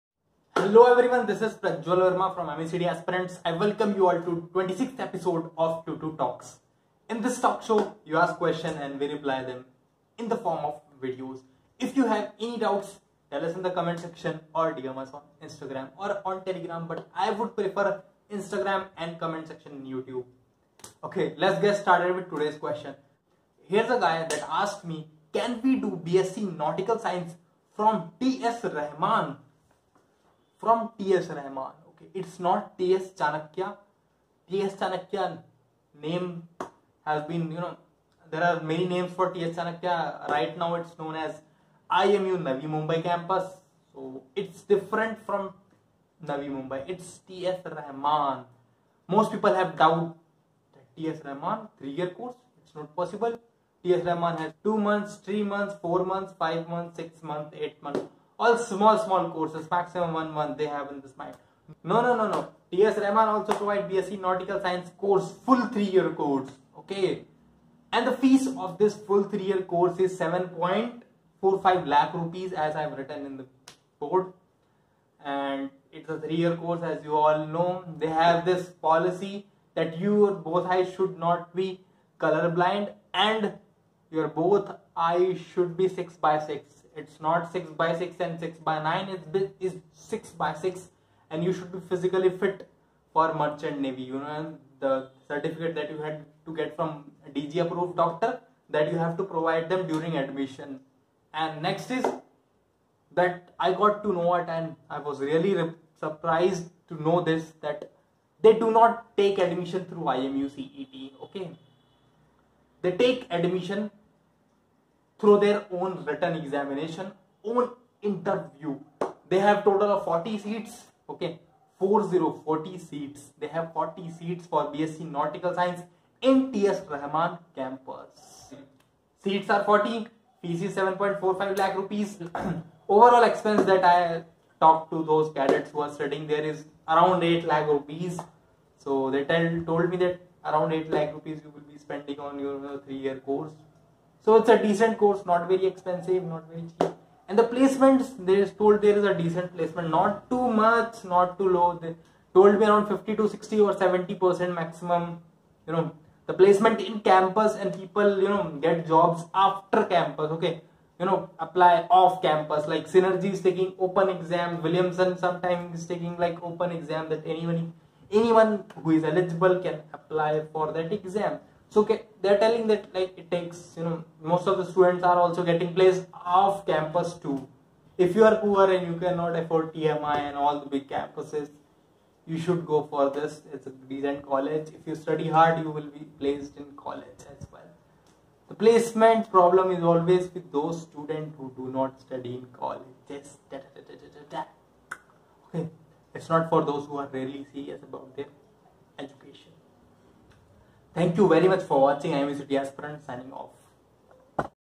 CAN WE DO BSC NAUTICAL SCIENCE IN TS RAHAMAN ?? || 22TALKS EPISODE 26 .
Imucetaspirants⚓
Hey everyone this channel is created to provide you free study material for IMU CET , updates , meet amazing people, free mock tests.